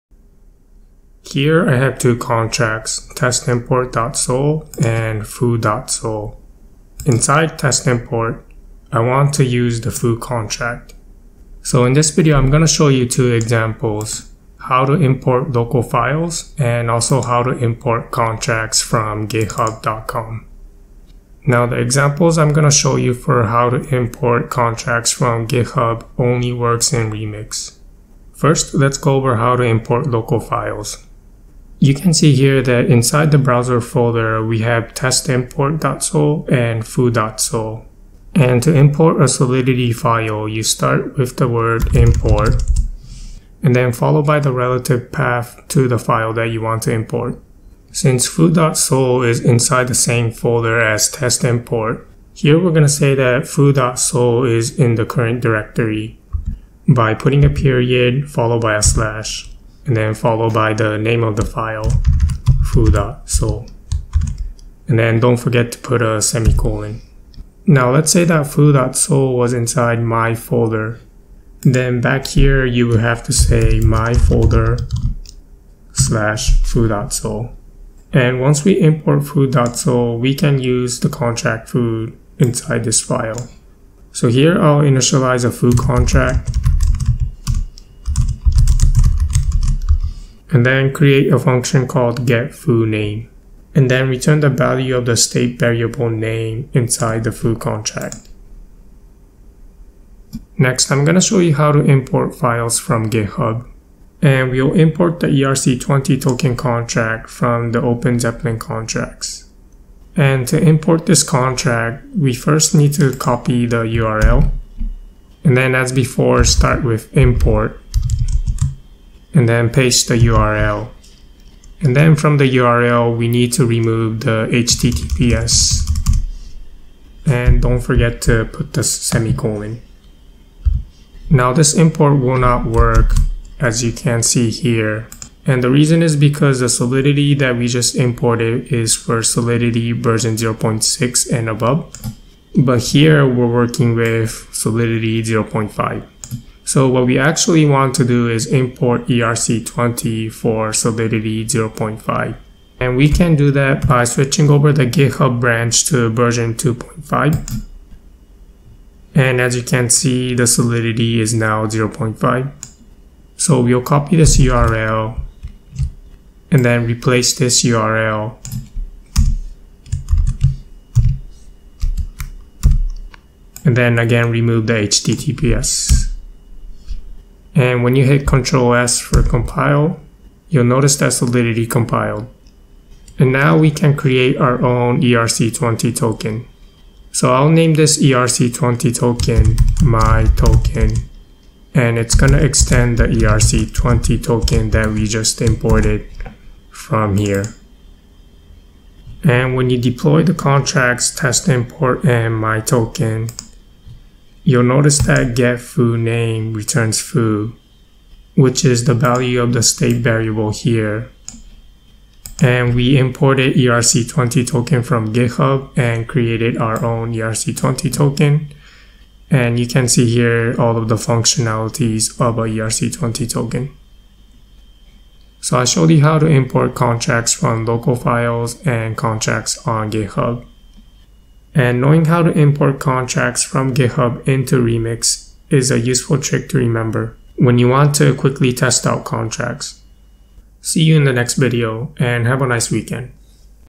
Learning Solidity (0.5) - Import
You can import other Solidity files into a contract by using the "import" keyword followed by the relative path to the Solidity file that you want to import. Furthermore, in Remix you can directly import contracts from GitHub.